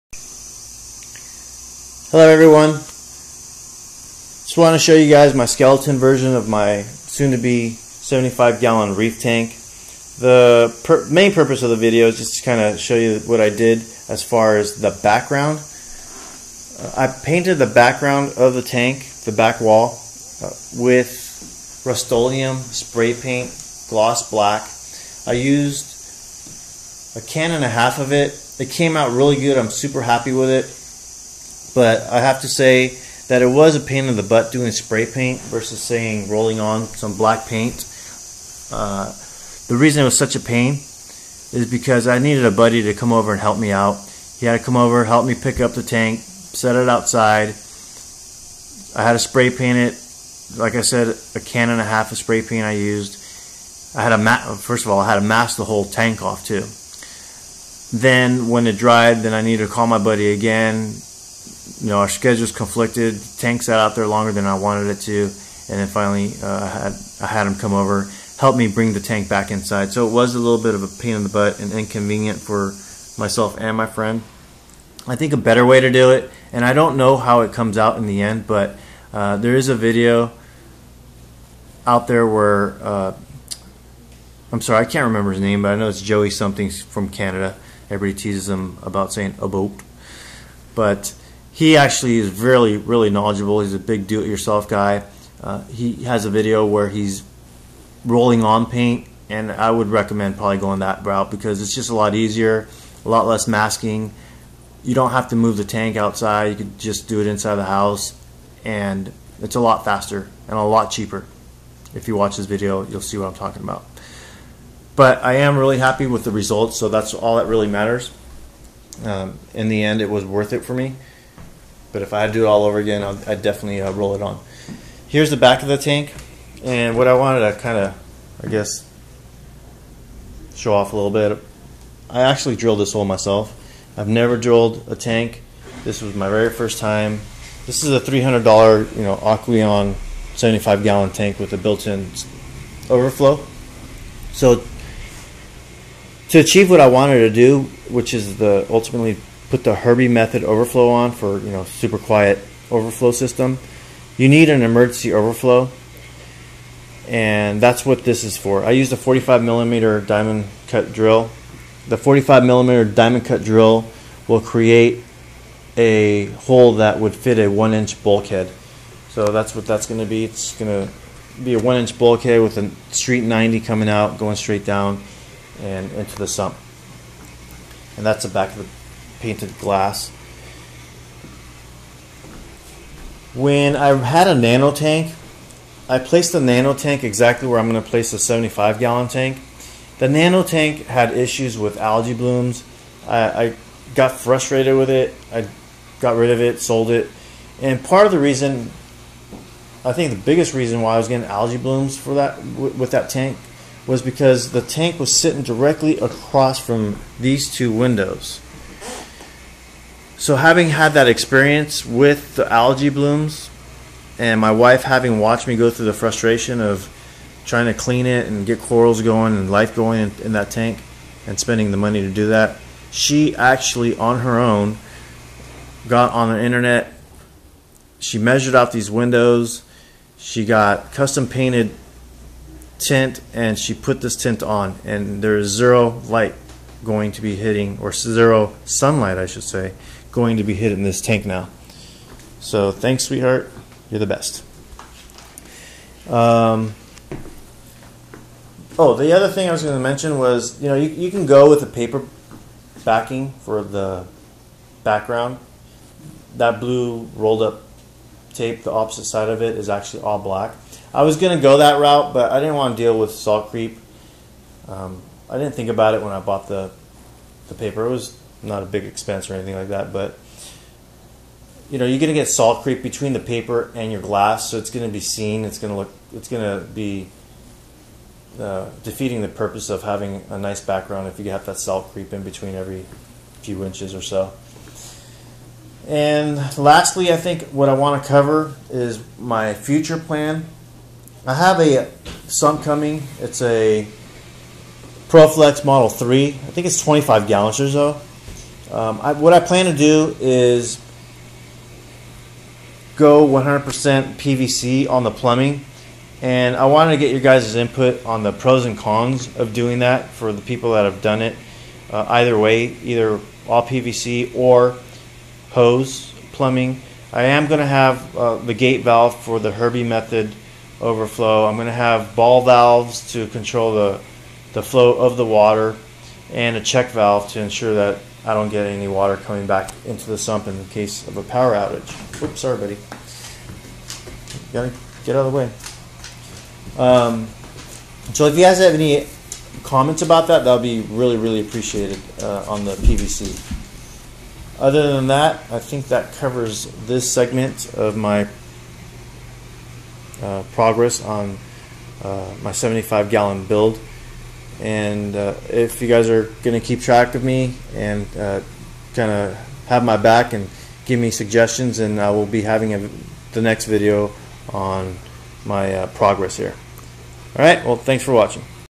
How To Paint Aquarium Background
This video explains how I spray painted the back of my 75 gallon saltwater aquarium with gloss black spray paint. The saltwater aquarium black background will give the future corals in the tank a dark aquarium background for their colors to "pop".